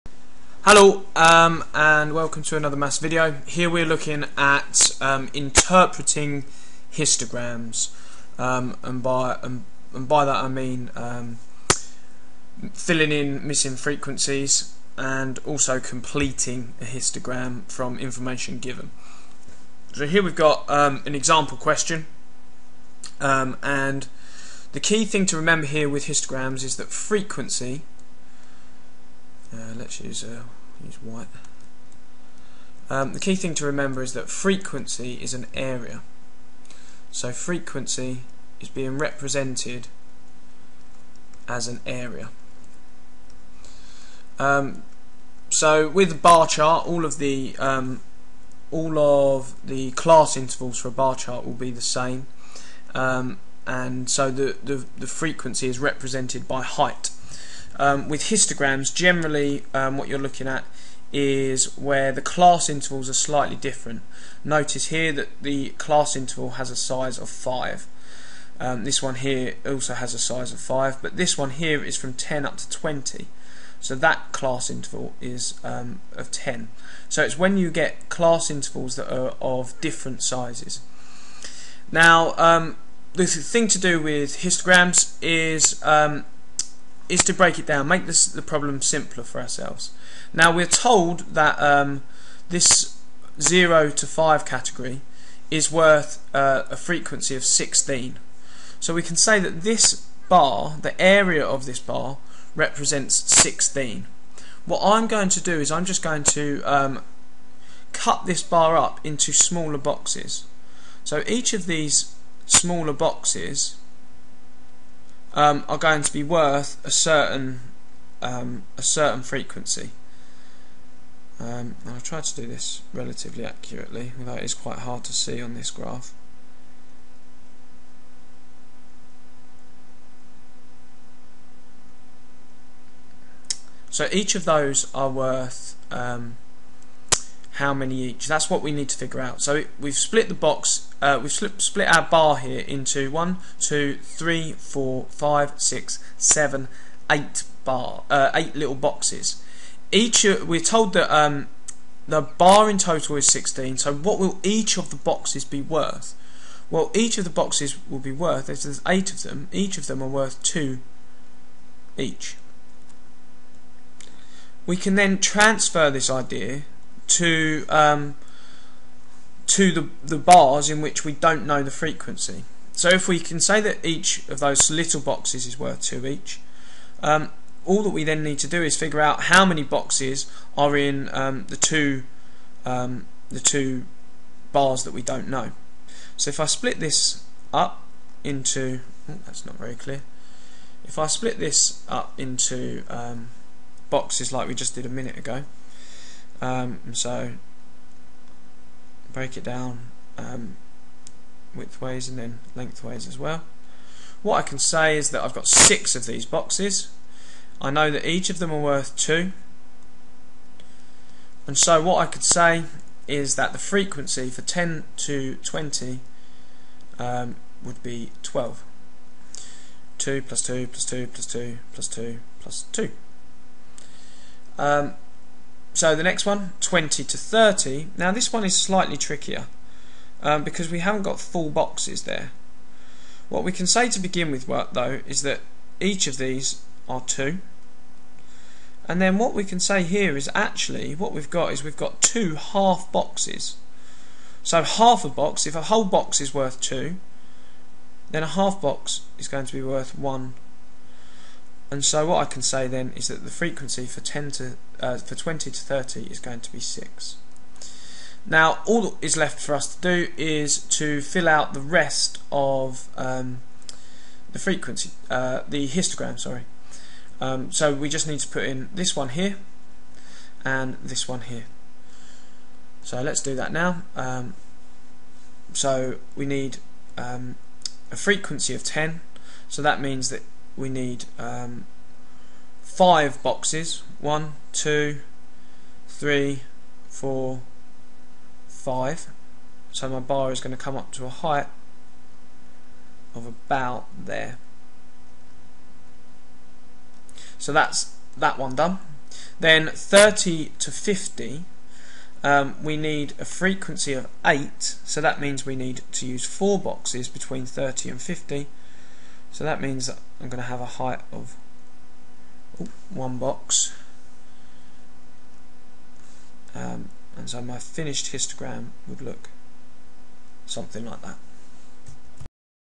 Histograms
Mr Jeffery taking a look at interpreting histograms.